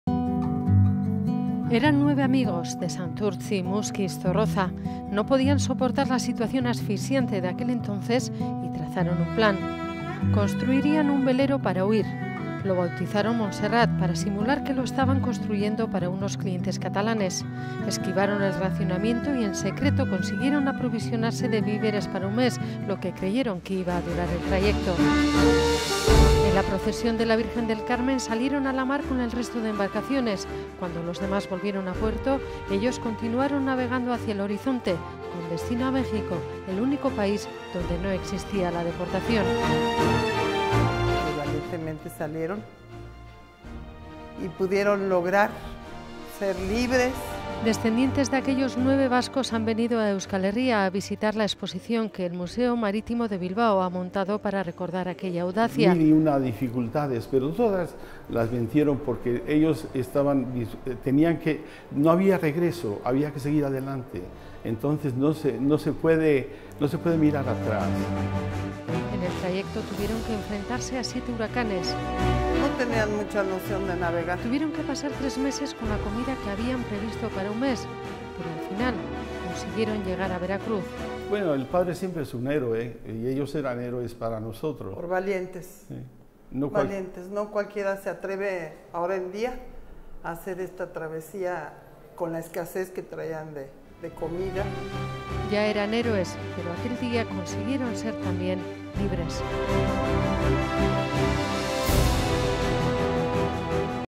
Exposición "Montserrat" en busca de la libertad (Eitb)
Video resumen de la exposición del Museo Marítimo "Montserrat" en busca de la libertad (gentileza de Eitb)